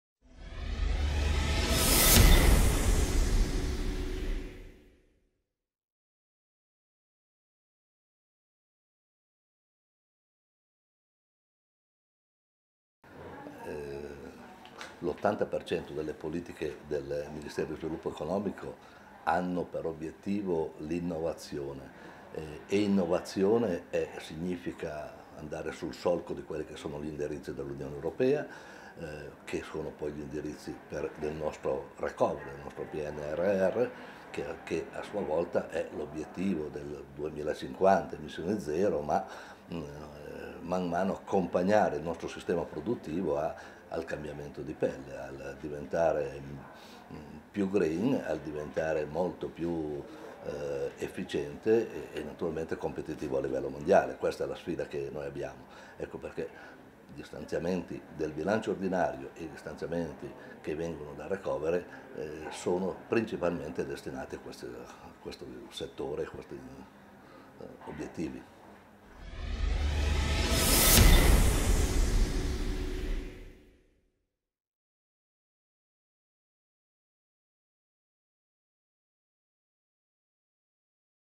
L'impegno del MiSE per la decarbonizzazione - Intervista al Viceministro Gilberto Pichetto Fratin
Accompagnare il sistema produttivo del Paese nel percorso verso la decarbonizzazione, preservandone la competività a livello mondiale. È la sfida a cui è chiamato il Ministero dello Sviluppo Economico. Ce ne parla il Viceministro Gilberto Pichetto Fratin.